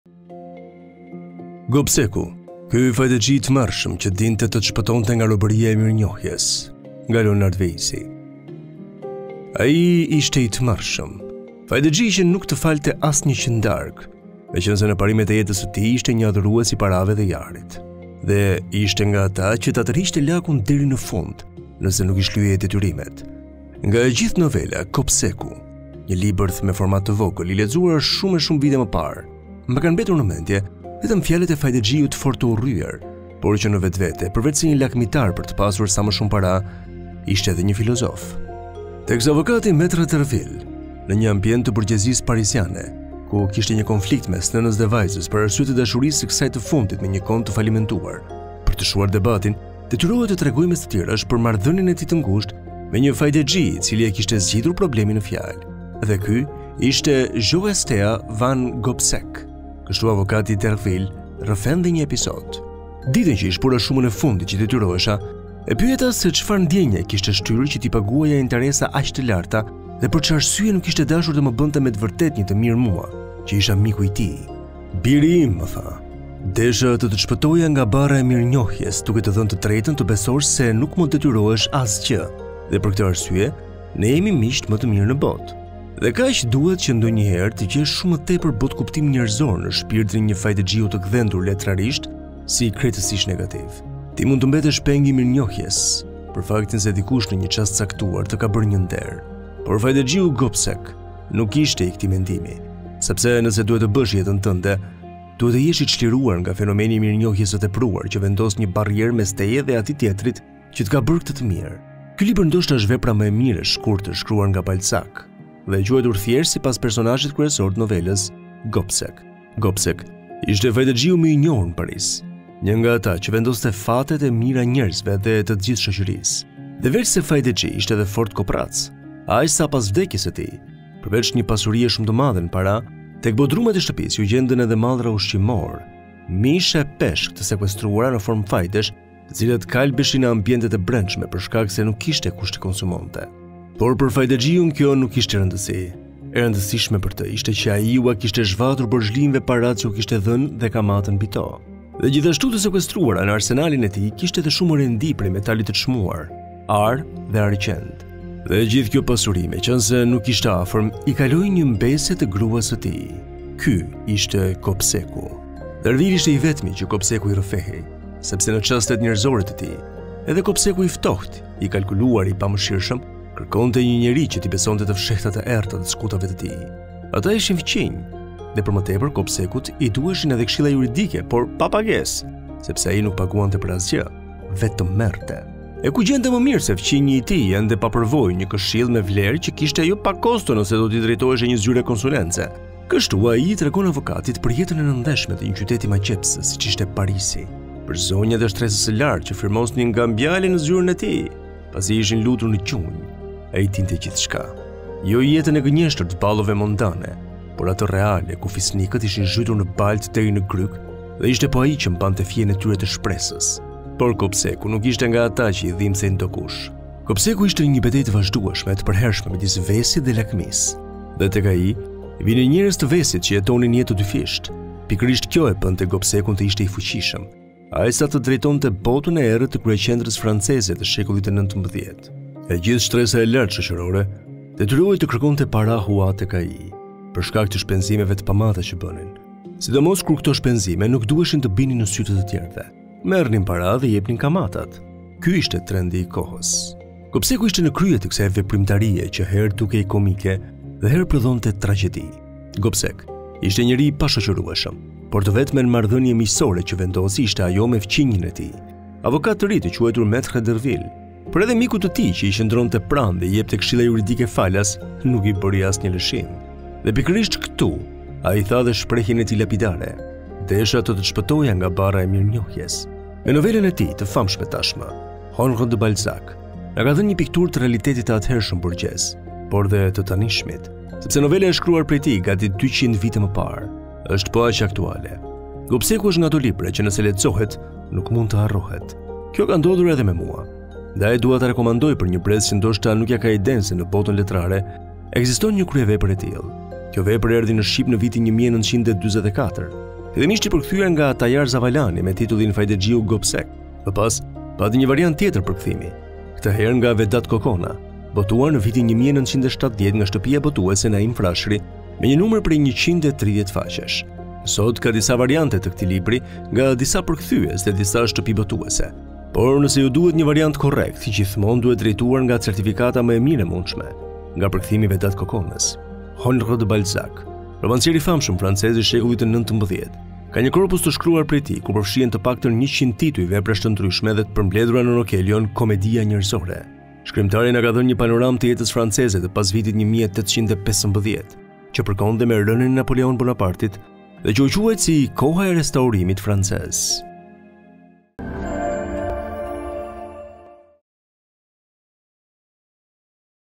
Gobseku,ky fajdexhi i tmerrshëm që dinte të të shpëtonte nga robëria e mirënjohjes/Nga Leonard Veizi
Ai ishte i tmerrshëm. Fajdexhi që nuk të falte asnjë qindarkë, meqenëse dhe në parimet e jetës së tij ishte një adhurues i parave dhe i arit. Dhe ishte nga ata që ta tërhiqte lakun deri në fund nëse nuk i shlyeje detyrimet.
Nga e gjithë novela “Gobseku”, – një libërth me format të vogël, i lexuar shumë e shumë vite më parë, – më kanë mbetur në mendje vetëm fjalët e fajdexhiut fort të urryer, por që në vetvete, përveçse një lakmitar për të pasur sa më shumë para, ishte dhe një filozof.
Teksa avokati Metre Dervil, – në një ambient të borgjezisë parisiene, ku kishte një konflikt mes nënës dhe vajzës për arsye të dashurisë së kësaj të fundit me një kont të falimentuar, – për të shuar debatin detyrohet të tregojë mes të tjerash për marrëdhënien e tij të ngushtë me një fajdexhi i cili e kishte zgjidhur problemin në fjalë. Dhe ky ishte Zho-Ester van Gobsek. Kështu avokati